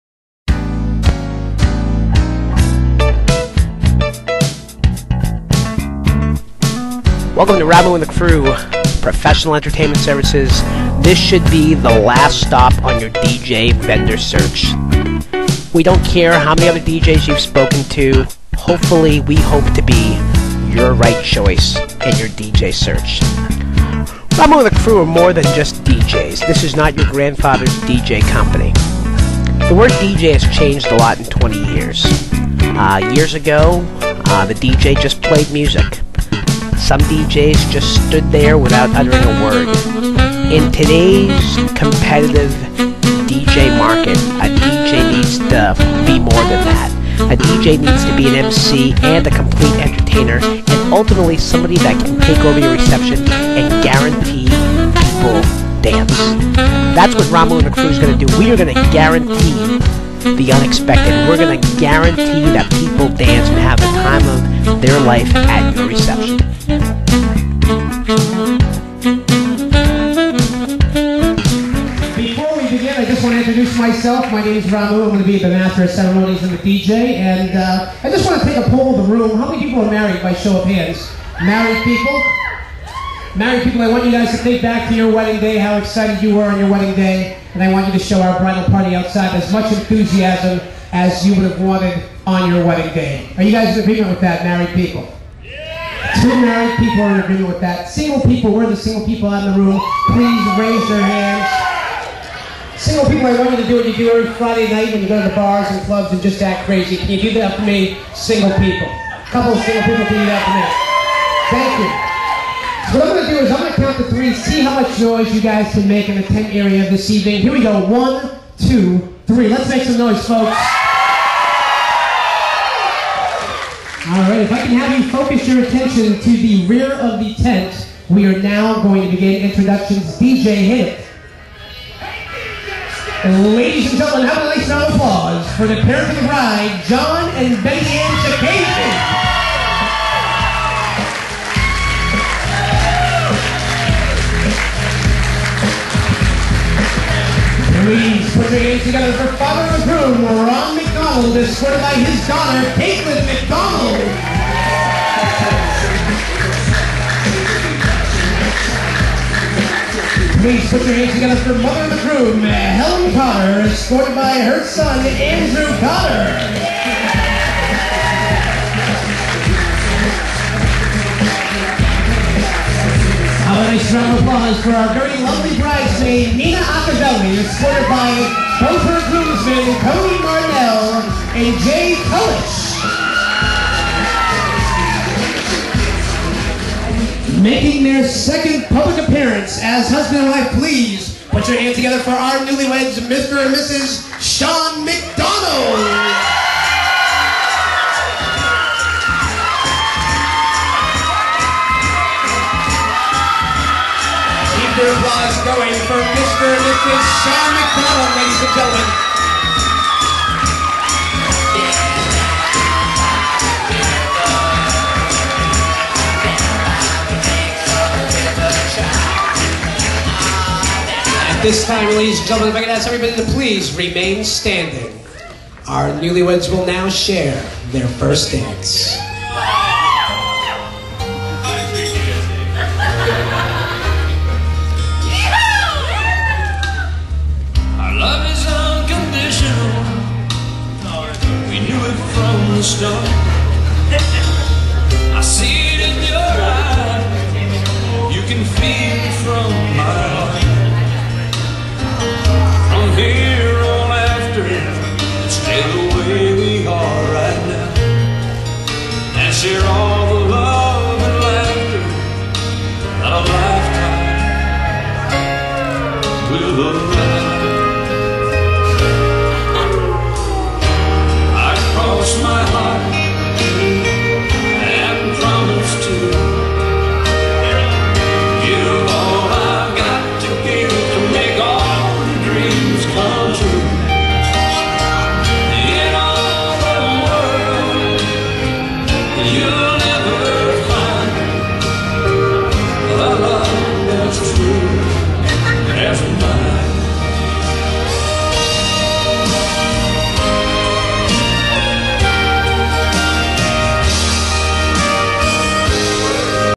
Ra-Mu and the Crew Promo pt 1
Promotional Video for Ra-Mu and the Crew Professional Entertainment and DJ Services. Operating out of Boston, and Providence, and service all of New England and Upstate New York since 1998.